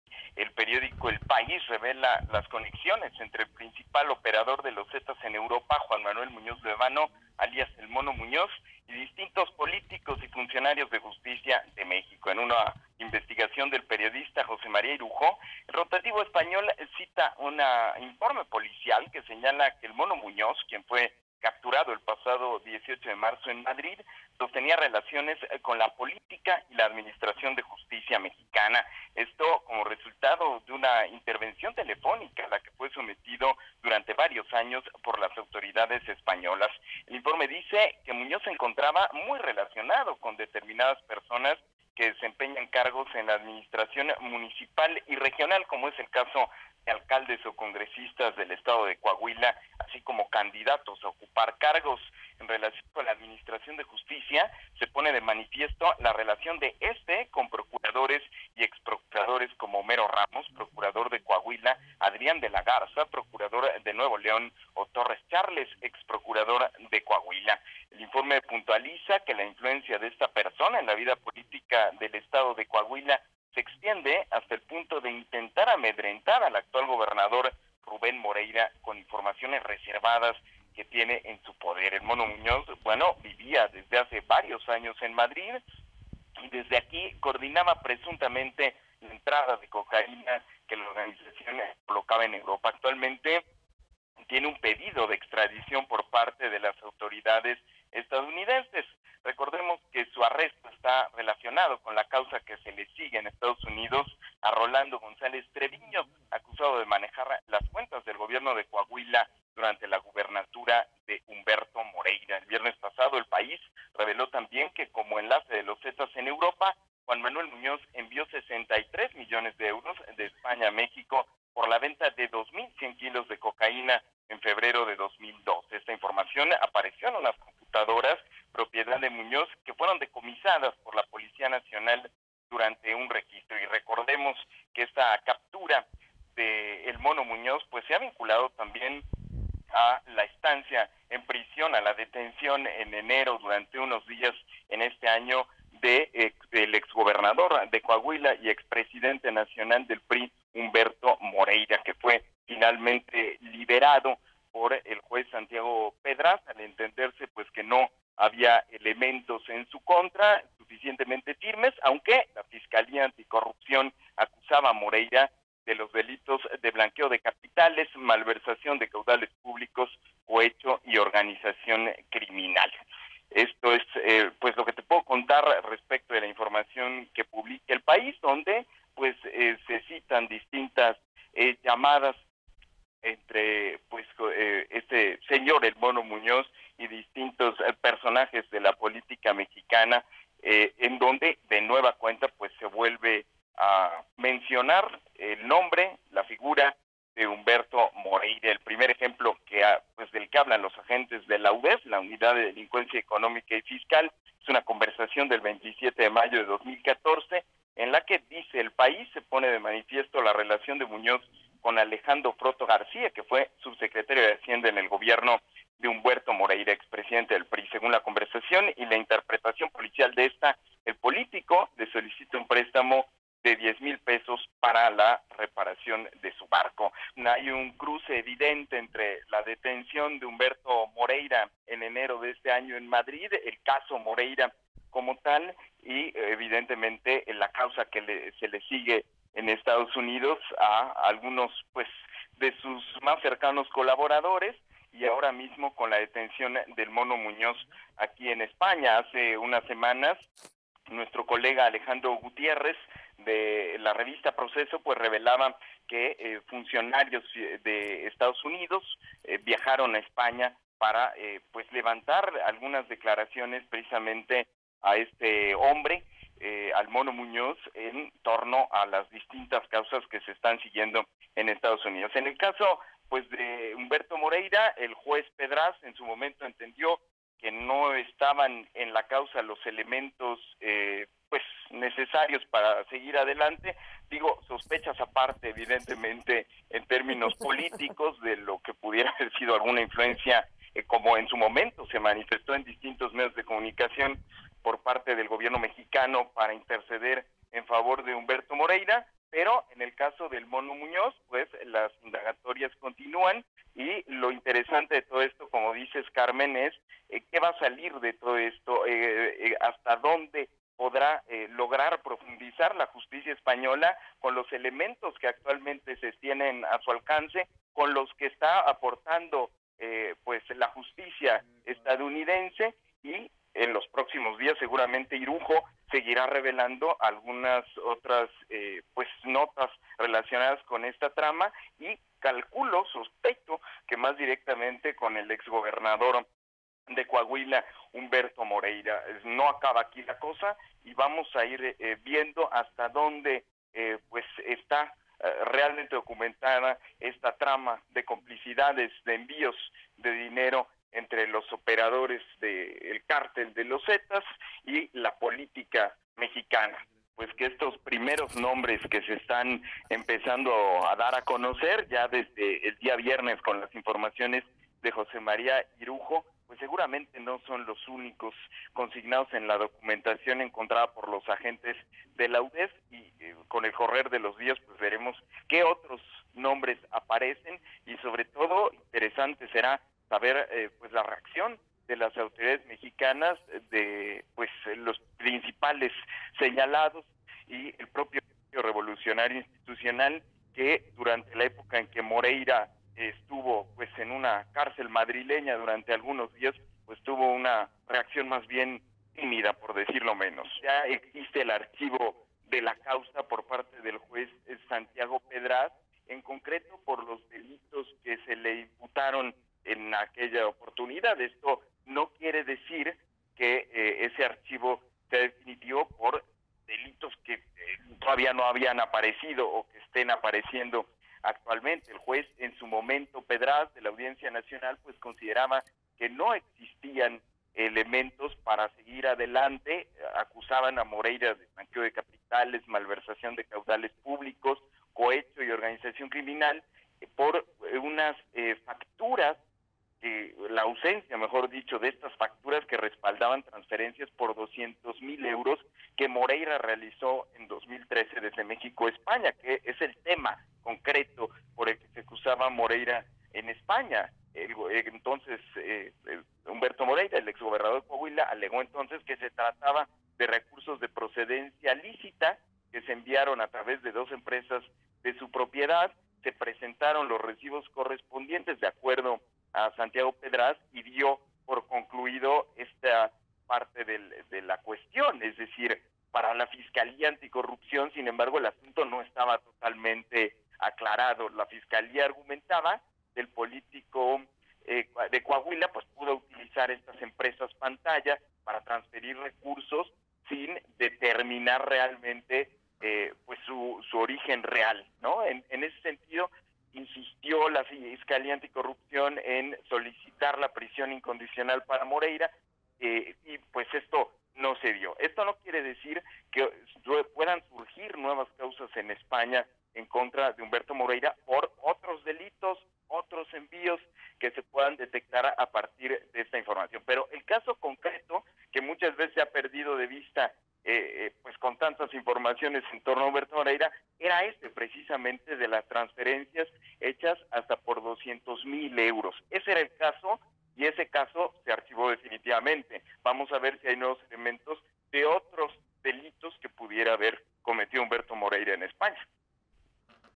Se cruza el caso Moreira con detención de enlace de ‘Los Zetas’: Manuel de Santiago
El periodista en España, Manuel de Santiago, detalló la investigación sobre Los Zetas en Europa y sus vínculos con políticos mexicanos, publicada en dos partes en el diario 'El País'. Recordó que investigaciones previas en Madrid conducieron hasta el ex gobernador Humberto Moreira, quien fue detenido y liberado en enero pasado. Juan Manuel Muñoz Luévano, alias Mono Muñoz, presunto enlace de Los Zetas en Europa, es el personaje al que se le ha seguido la pista en España y fue detenido el pasado 18 de marzo. "Hay un cruce evidente entre la detención de Humberto Moreira... y la causa que se le sigue en Estados Unidos a algunos de sus más cercanos colaboradores y ahora con la detención de El Mono Muñoz aquí en España", puntualizó.